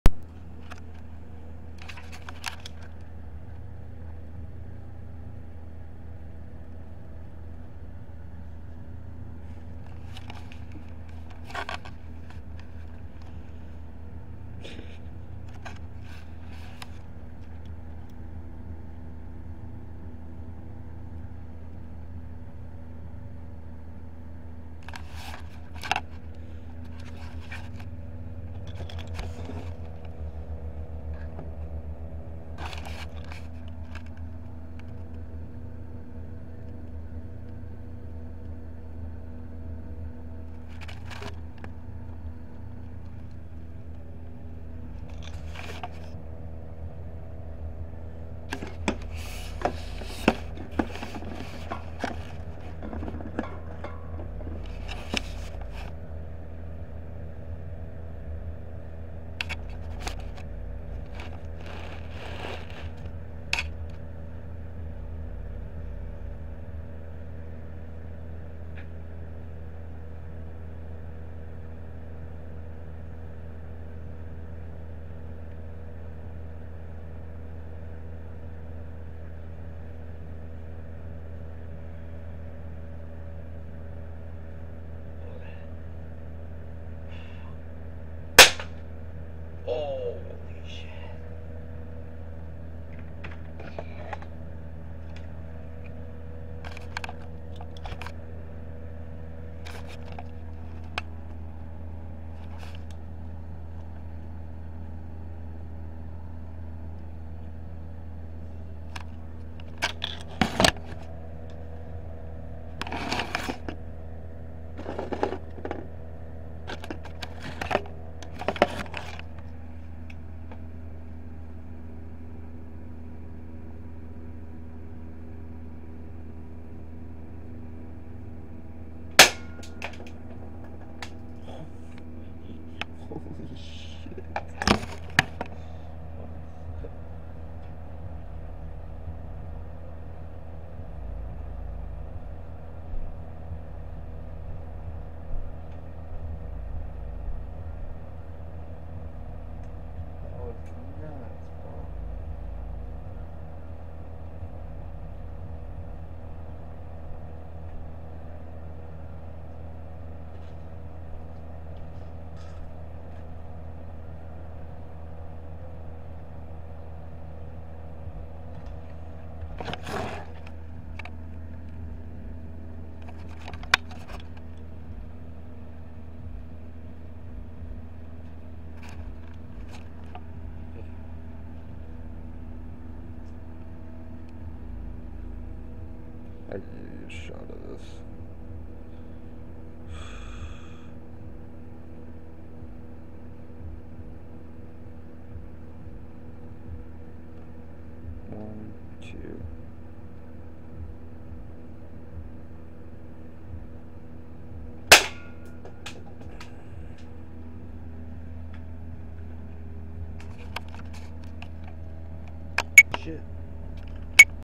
MY GIRLFRIEND showcases a FEW OF MY BB GUNS, I LOVE THIS GIRL!
DON'T TRY THIS AT HOME! SEND IT! GAINER SQUAD! IF THIS GETS 100K LIKES I'LL SPEND IT ON A DIAMOND RING!